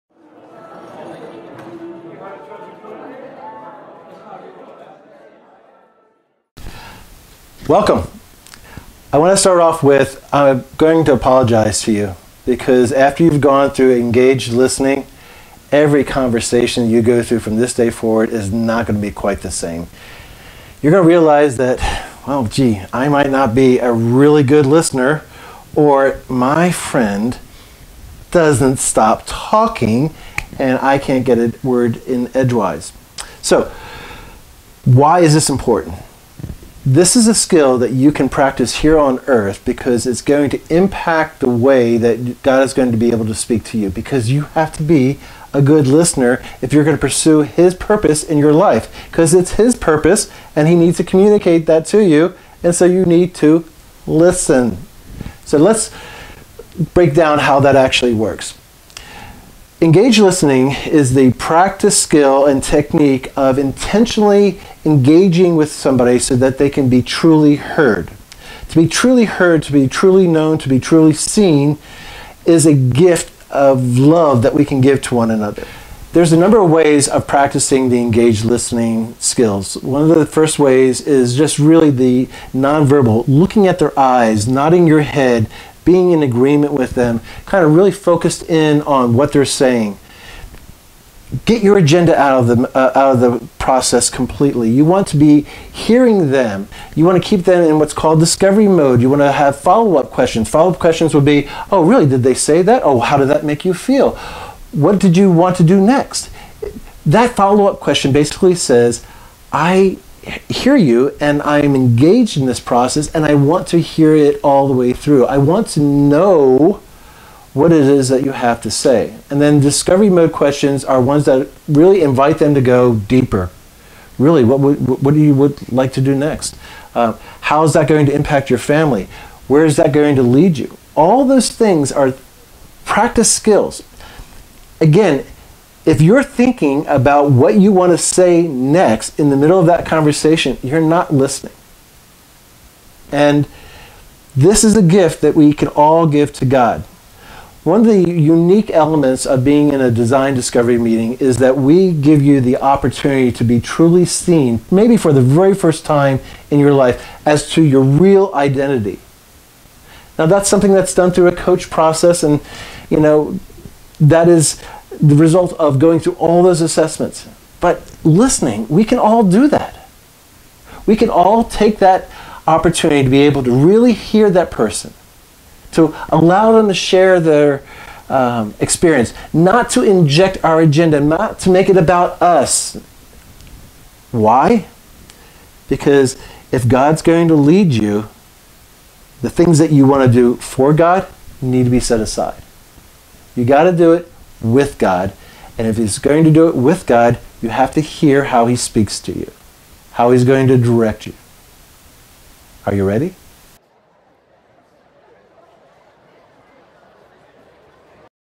BrushFires Workbook ● Engaged Listening - Chapter 6
To be truly heard, to be truly known and to be truly seen in your true identity, is a real expression of love. Engaged Listening is one of the hardest, but the most valuable tools for relationships, that we can impart to you. Engaged Listening is a discipline of conversation that first and foremost, requires us to put aside our agendas. It is technique and practiced skill to honestly engage in conversation for the primary purpose to let the speaker know they are being truly heard. It is asking questions and giving responses in such a way, that the speaker will be invited to share beyond the superficial level. Engaged Listening is a gift you have to give to your fellow human beings.